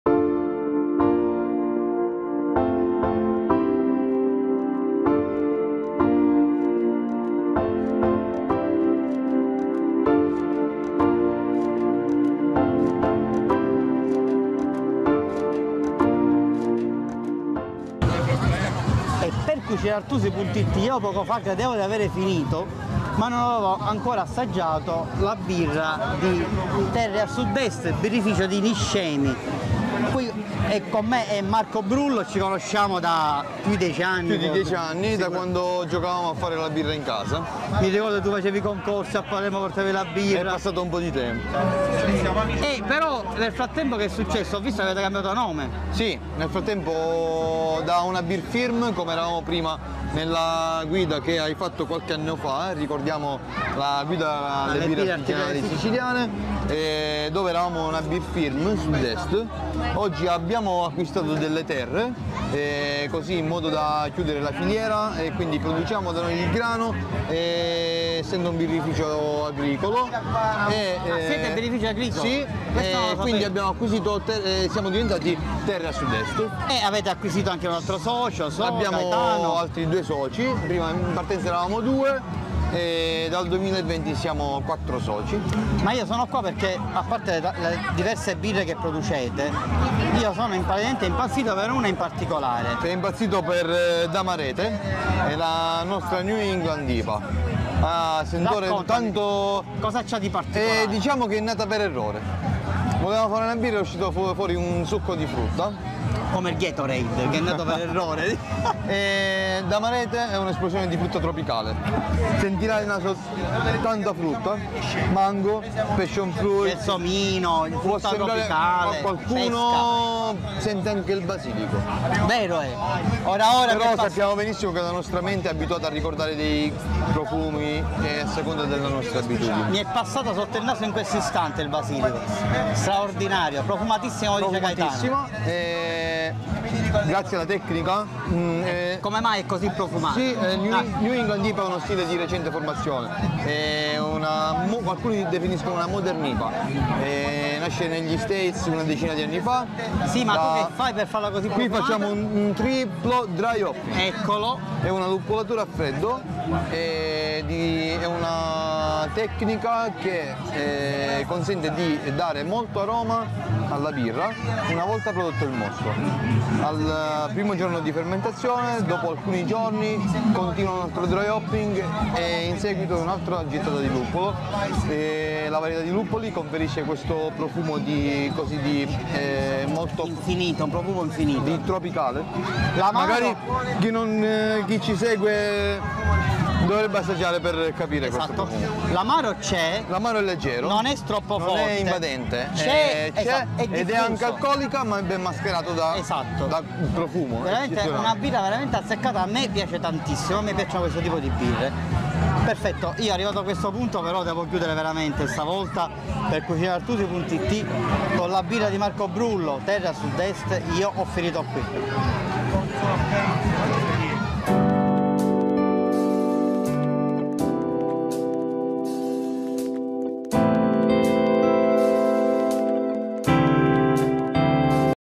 La Neipa di Terre a Sud Est
Maurizio Artusi recensisce e intervista per CucinArtusi.it
Articolo completo:

Le magliette di Maurizio Artusi sono a cura del Ricamificio La Nuvola di Palermo